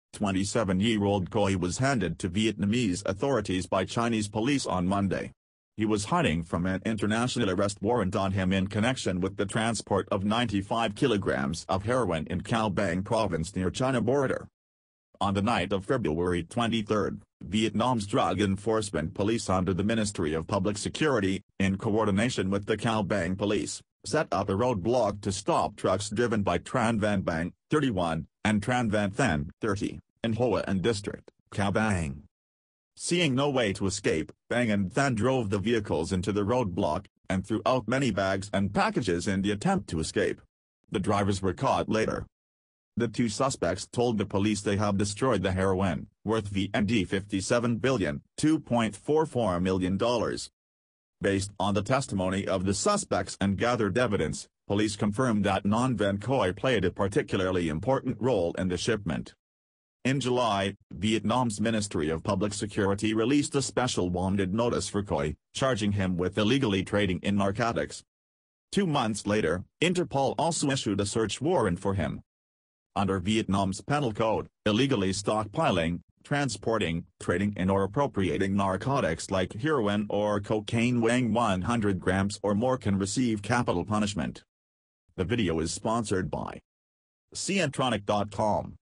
China sends Interpol sought drug dealer back to Vietnam
27-year-old Khoi was handed to Vietnamese authorities by Chinese police on Monday. He was hiding from an international arrest warrant on him in connection with the transport of 95 kilograms of heroin in Cao Bang Province near China border.
On the night of February 23, Vietnam’s Drug Enforcement Police under the Ministry of Public Security, in coordination with the Cao Bang police, set up a roadblock to stop trucks driven by Tran Van Bang, 31, and Tran Van Thanh, 30, in Hoa An District, Cao Bang.
Seeing no way to escape, Bang and Thanh drove the vehicles into the roadblock, and threw out many bags and packages in the attempt to escape. The drivers were caught later.
The two suspects told the police they have destroyed the heroin, worth VND57 billion ($2.44 million).
Based on the testimony of the suspects and gathered evidence, police confirmed that Nong Van Khoi played a particularly important role in the shipment.
In July, Vietnam’s Ministry of Public Security released a special wanted notice for Khoi, charging him with "illegally trading in narcotics."
Two months later, Interpol also issued a search warrant for him.
Under Vietnam's Penal Code, "illegally stockpiling, transporting, trading in or appropriating narcotics" like heroin or cocaine weighing one hundred grams or more can receive capital punishment.